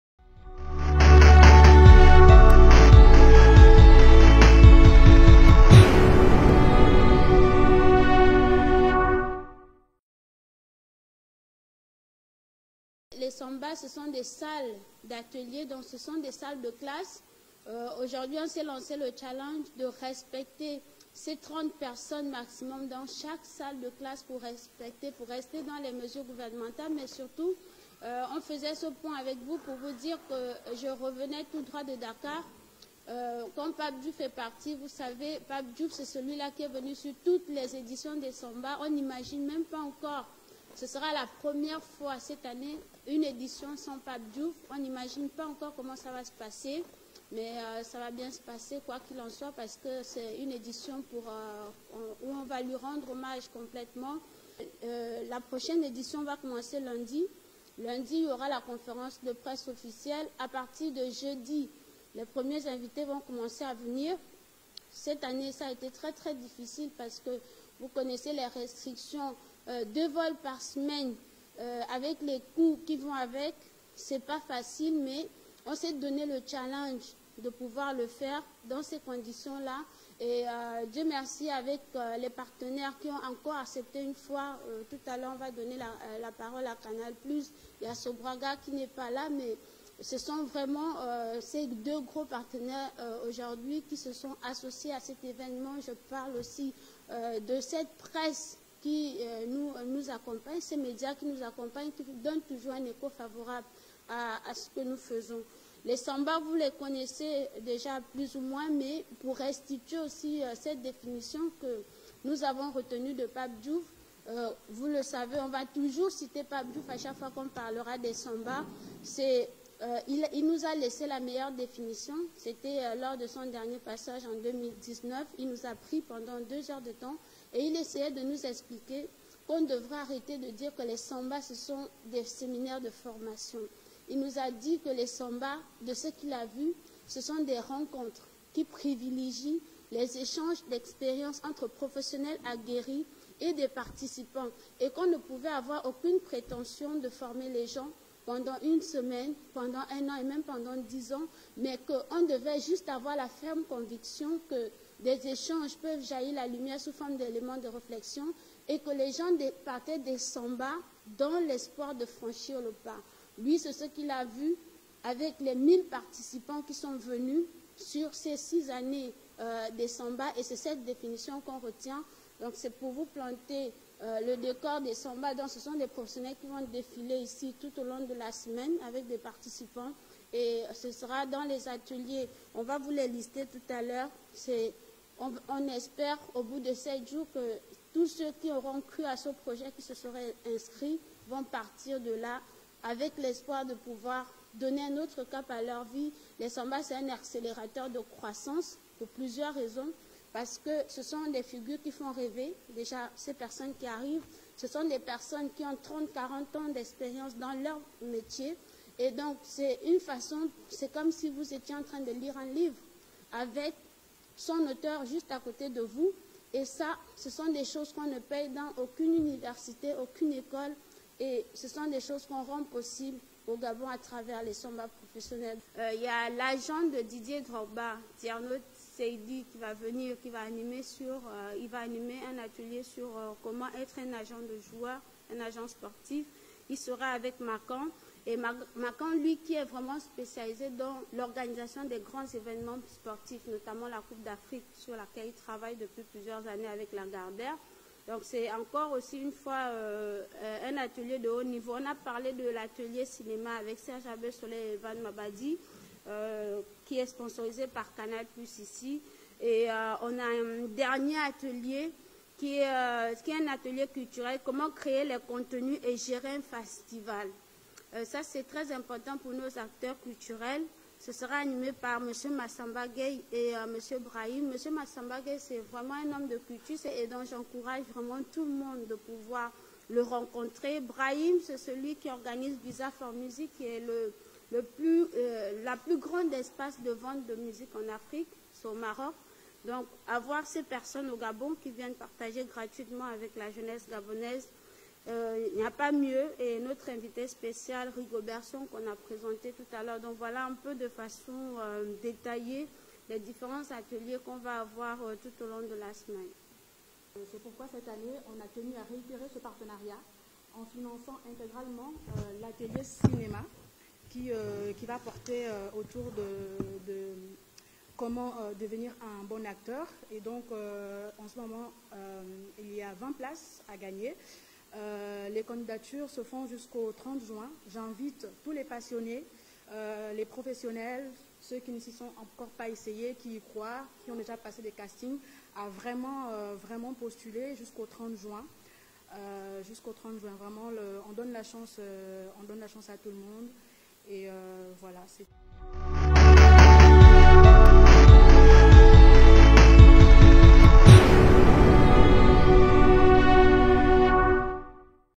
Gabon 30 06 2021/POINT DE PRESSE DE LA 7èmes ÉDITION DES SAMBAS PROFESSIONNELS:RIGOBERT SONG ATTENDU
Gabon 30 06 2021 / POINT DE PRESSE DE LA 7èmes ÉDITION DES SAMBAS PROFESSIONNELS: RIGOBERT SONG ATTENDU A LIBREVILLE